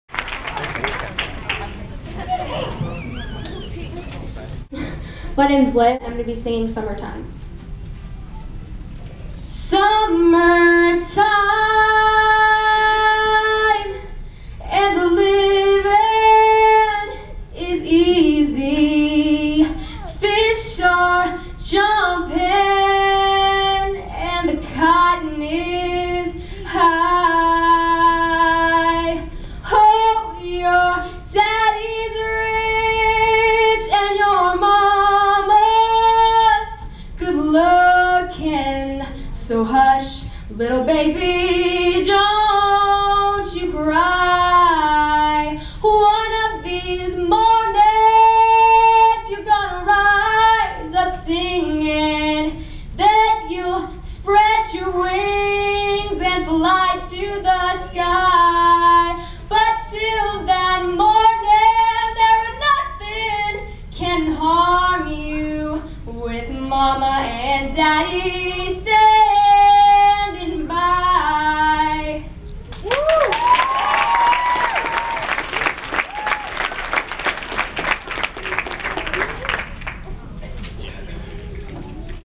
Audition
Audition I did for a talent agency thinger.. haha. Sorry for the random jump cut at the beginning. Had to cut out personal info. ;)